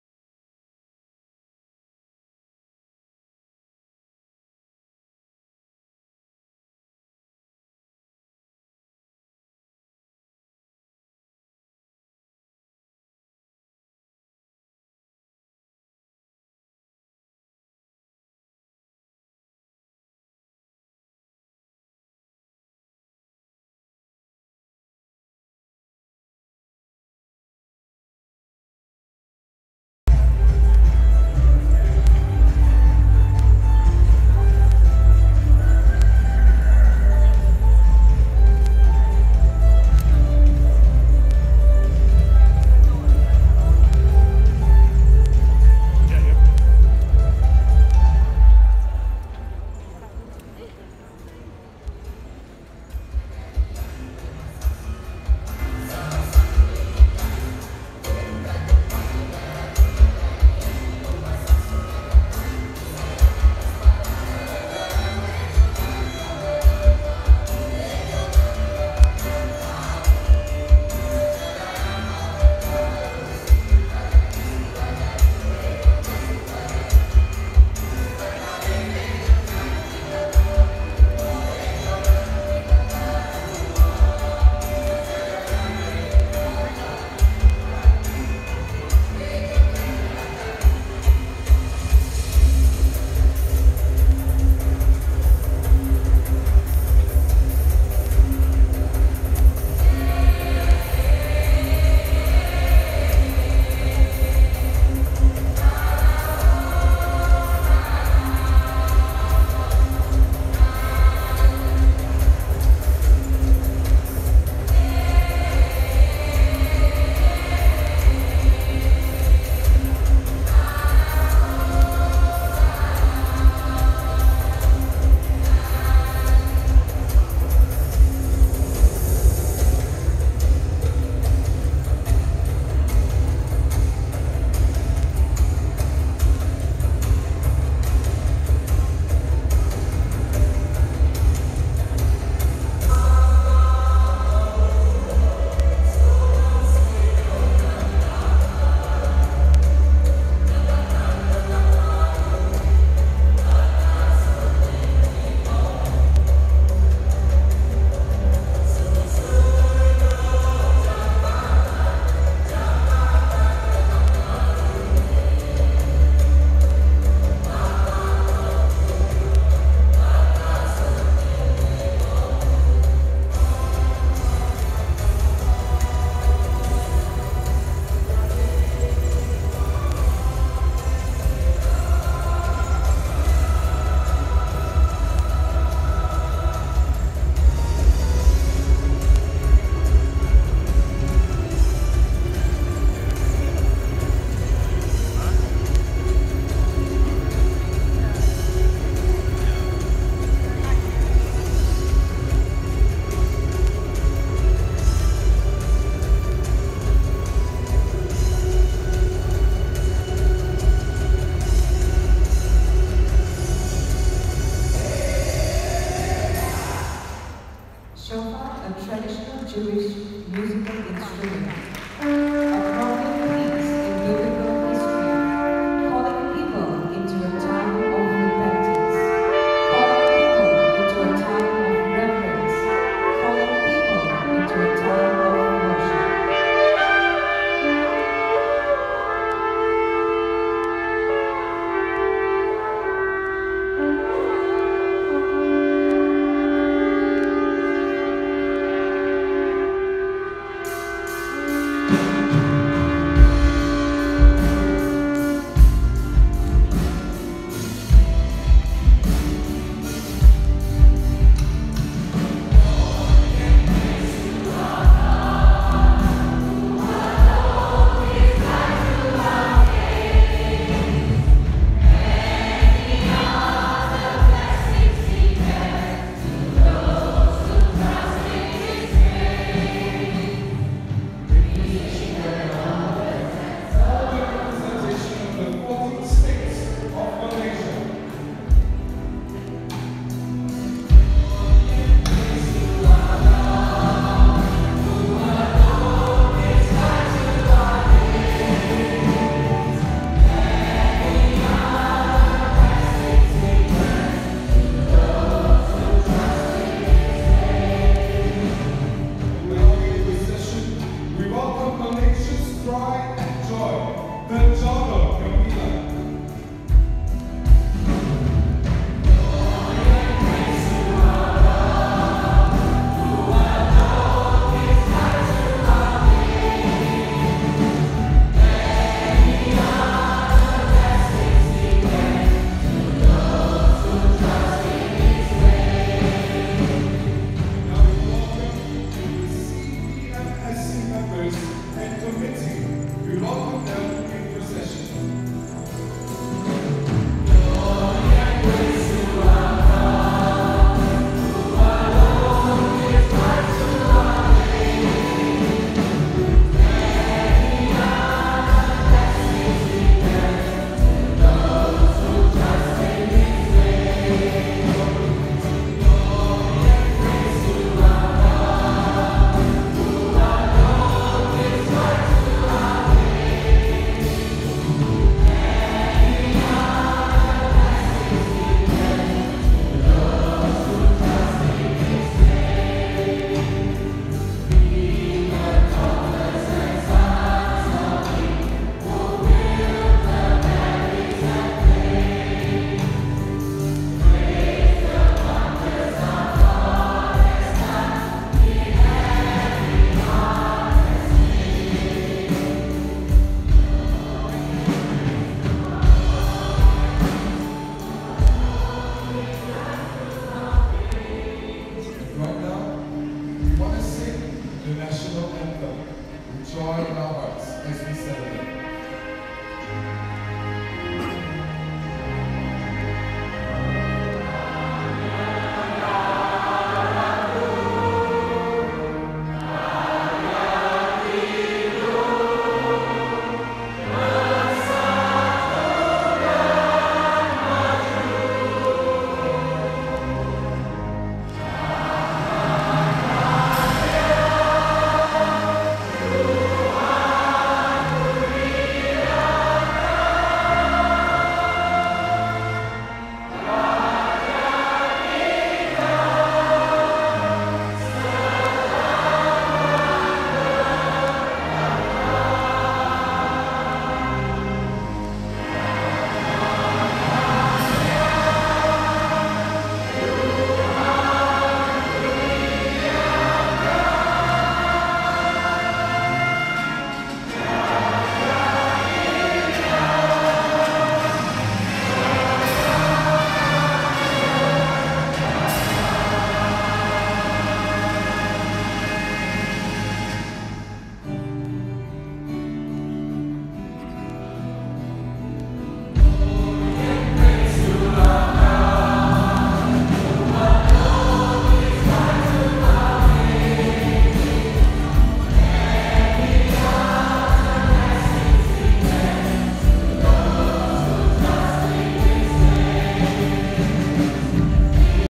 Golden Jubilee Celebration of the Malaysia Catholic Charismatic Renewal - Archbishop Julian Leow, DD
50th Golden Jubilee Celebration of the Catholic Charismatic Renewal in Peninsula Malaysia (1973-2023)
REBUILD - RESTORE - RENEW
"Behold I make all things new" Rev 21:5
Organized by: CHARIS Peninsula Malaysia Service of Communion (CPMSC)
Eucharistic Celebration by Most Rev. Julian Beng Kim, D.D. Archbishop of Kuala Lumpur
Venue: Church of Holy Family, Kajang, Selangor, Malaysia
Date: 31st August - 2nd September 2023